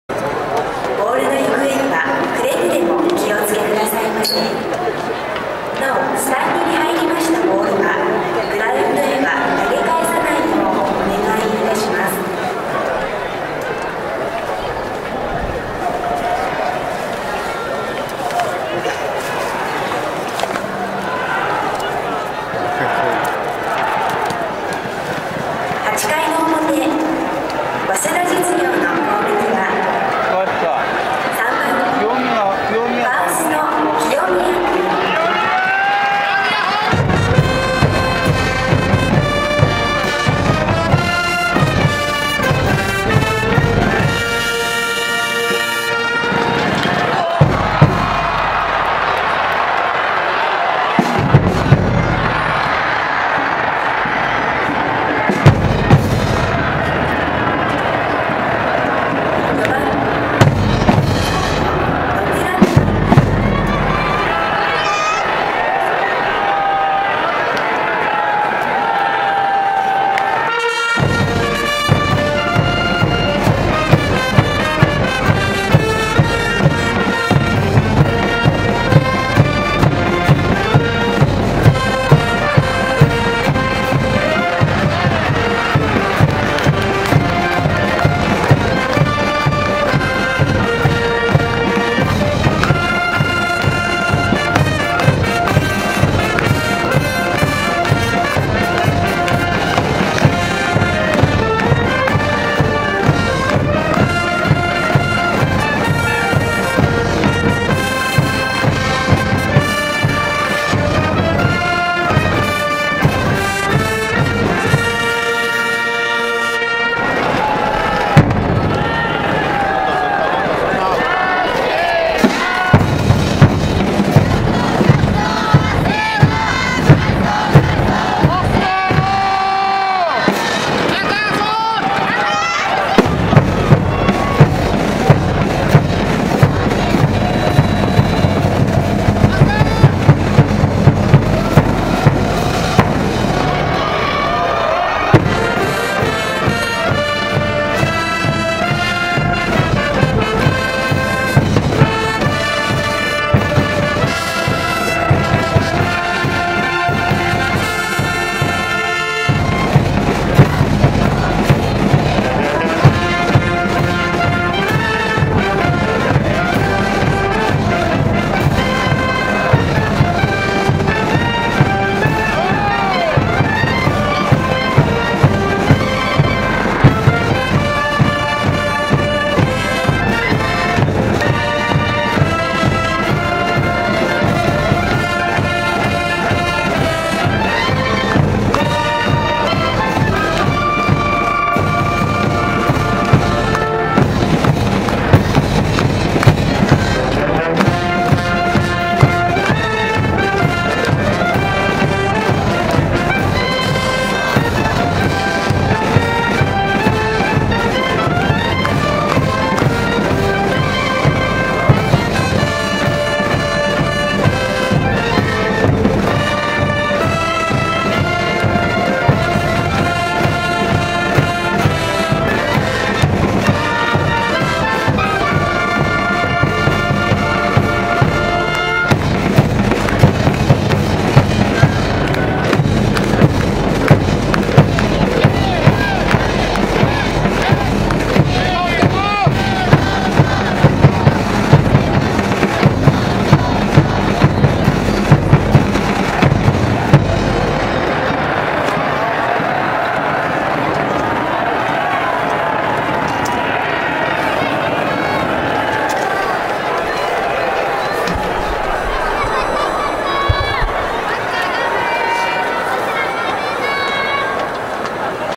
早稲田実－明徳義塾 ８回表
２０１７年３月２４日金曜
１１：１７～１３：４４（２時間２７分）

第８９回選抜高等学校野球大会１回戦
大会５日目　第２試合
早稲田実－明徳義塾

早稲田実　０１００００１０２　１　５ １１　０
明徳義塾　３０００００１００　０　４　 ８　１
延長１０回

・１塁側内野席「早稲田実」側から撮影
・充電池入れ替えの際、一部撮影出来ていません